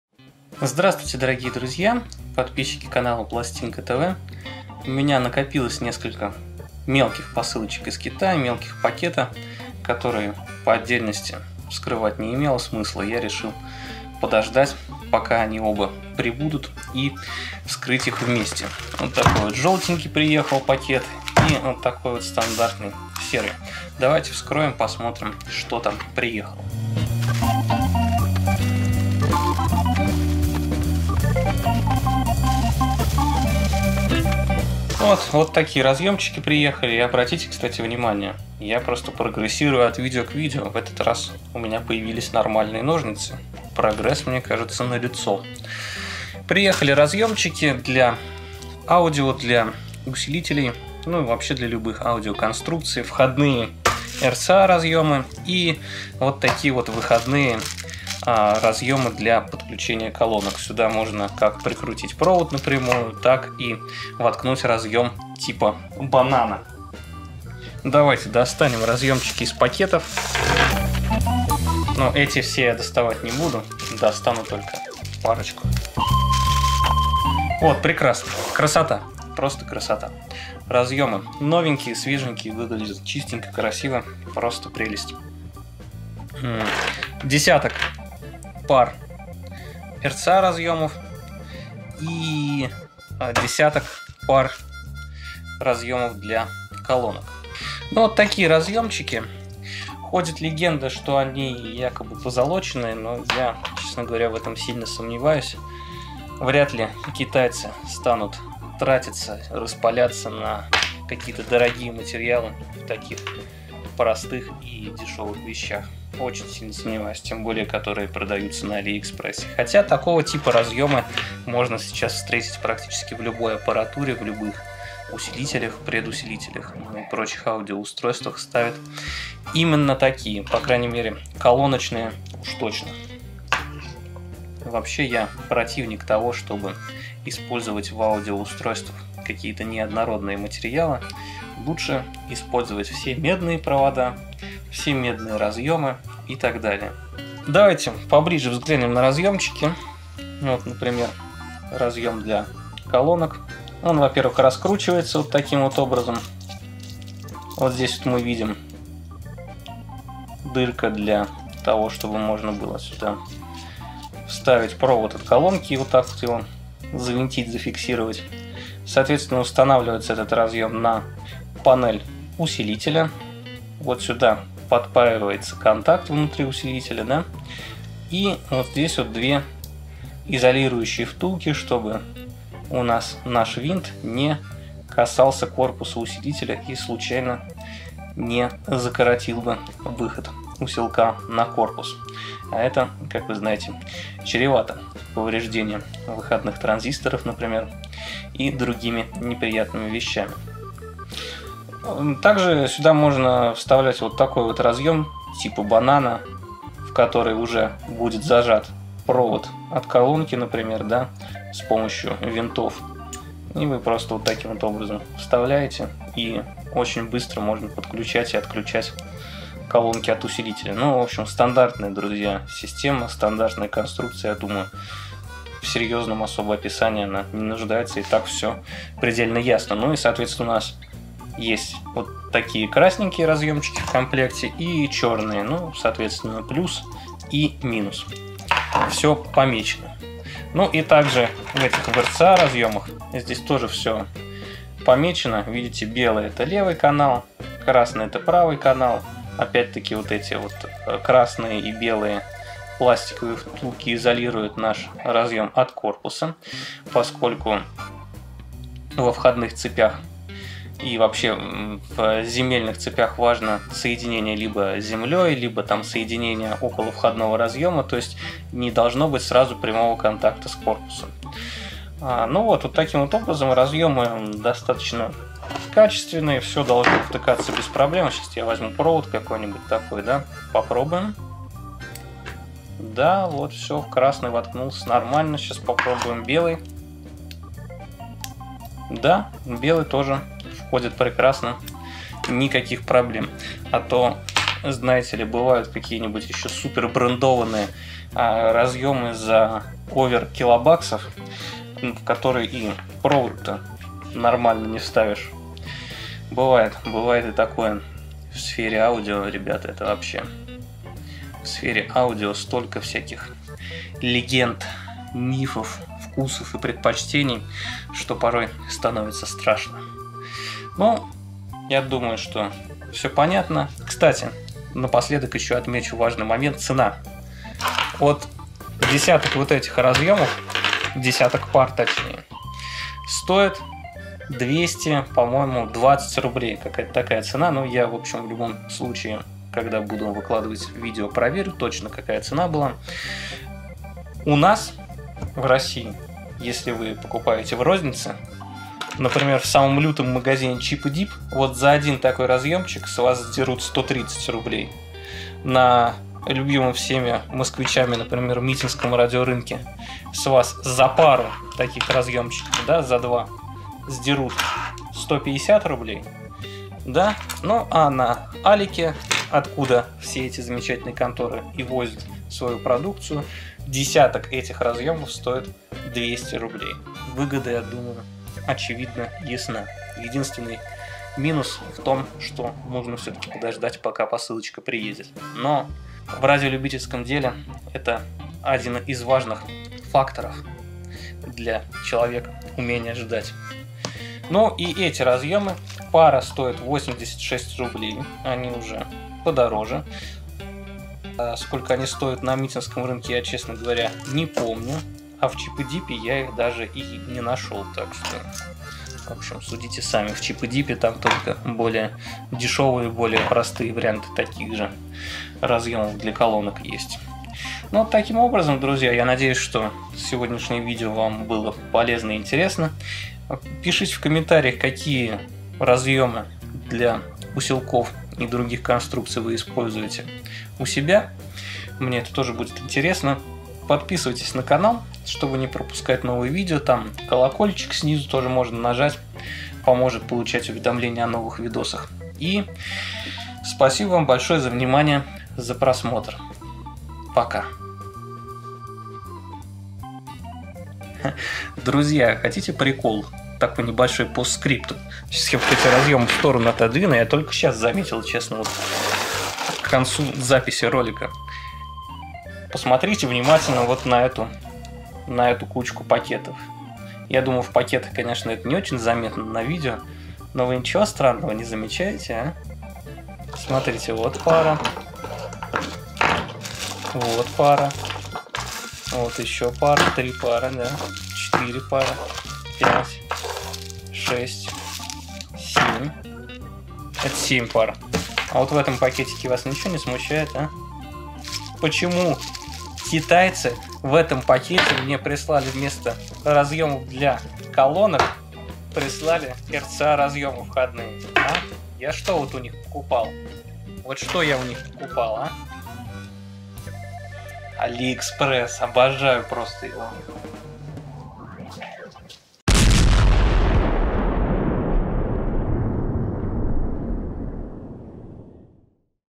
Бананы и тюльпаны
Распаковка посылки с китайского Алиэкспресс с разъемами RCA для линейного сигнала и винтовыми зажимами для колонок с возможностью подключения разъемов типа Banana. Китайцы мухлюют, досмотрите видео до конца.